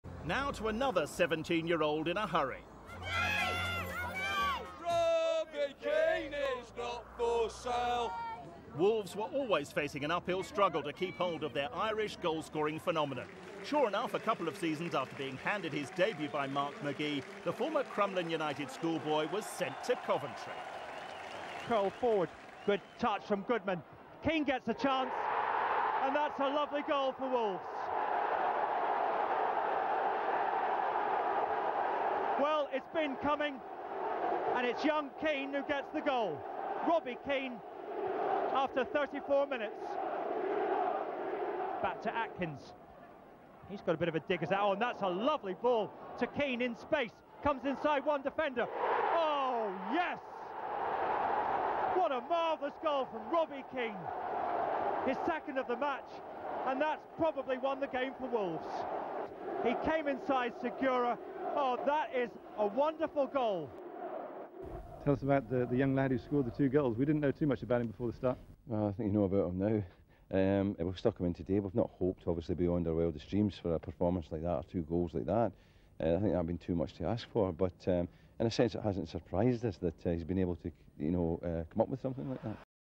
WOLVERHAMPTON WANDERERS FC V NORWICH CITY FC– ROBBIE KEANE DEBUT – 9TH AUGUST 1997 –CARROW RD
Robbie Keane scores the twice on his first team debut for Wolverhampton Wanderers FC against Norwich FC in a 0-2 win. This Division One match was played at Carrow Rd, Norwich on the 9th August 1997. Attendance: 17,230